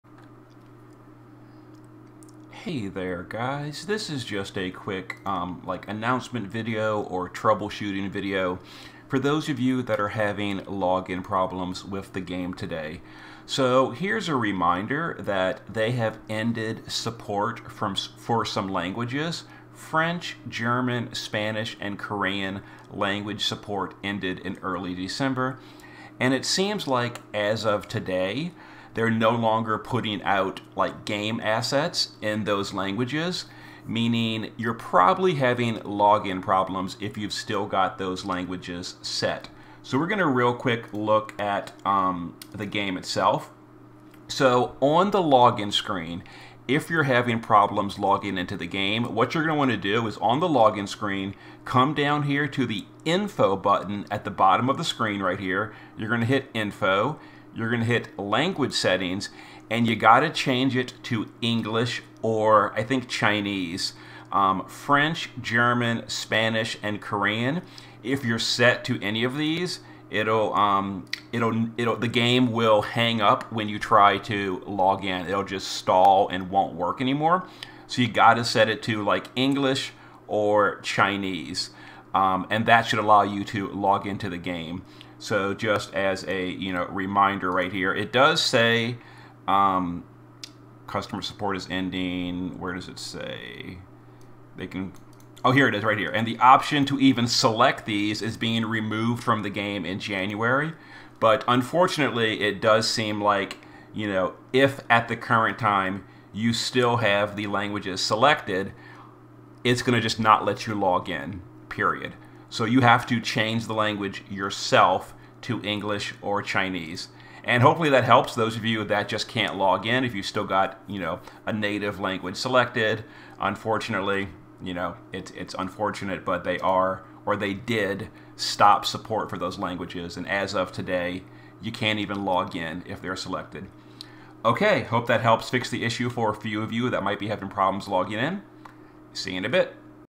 If you can't login, check your language setting!
Quick tip for how to fix unable to login problems today